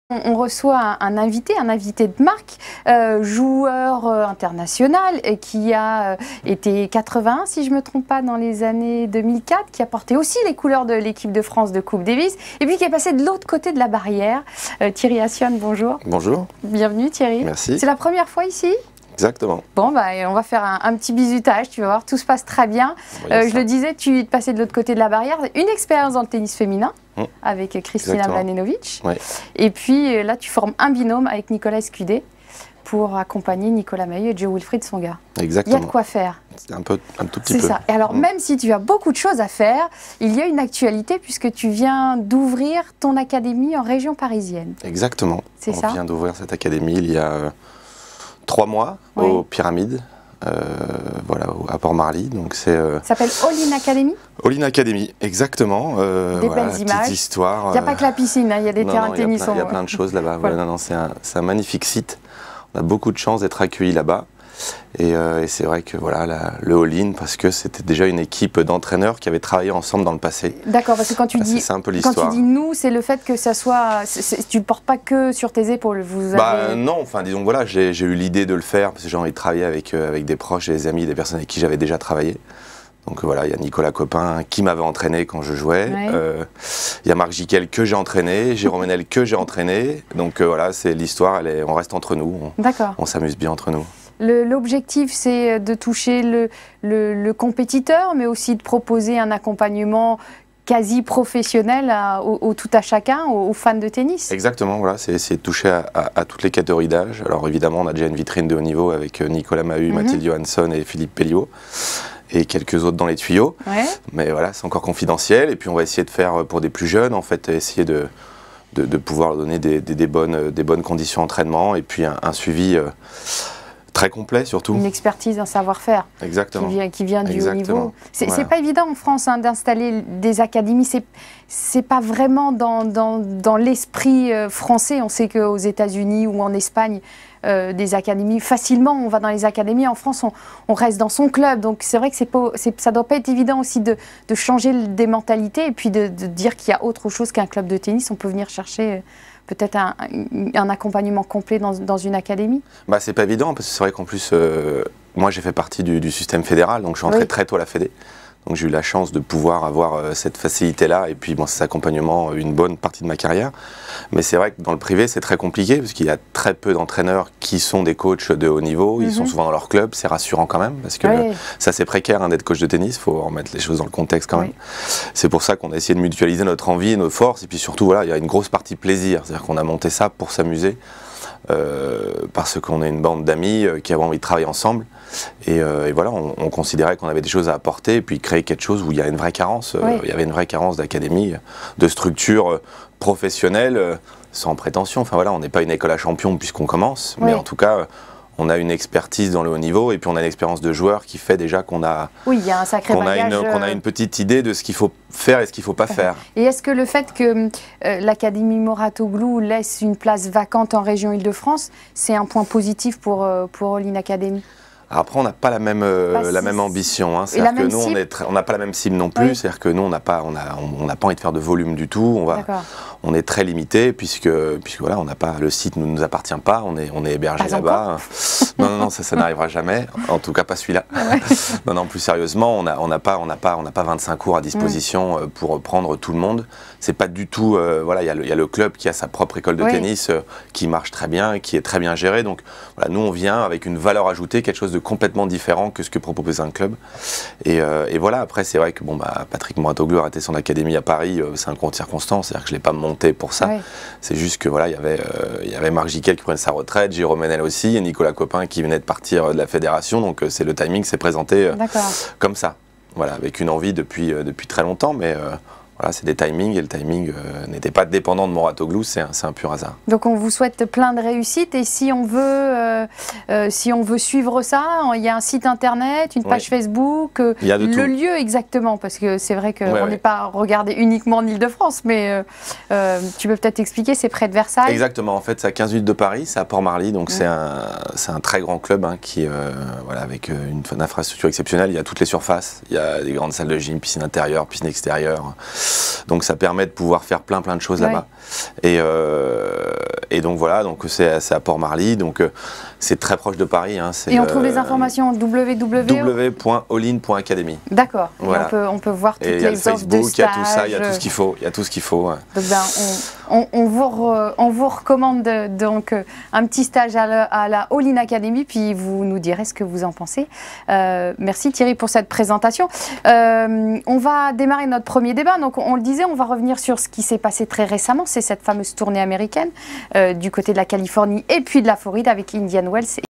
All In Academy- Ma chaine Sport 7 Avril 2015
Thierry Ascione, ex 81ème ATP, entraineur de J.W Tsonga
et Nicolas Mahut ouvre son Académie de Tennis, la All in Academy aux Pyramides.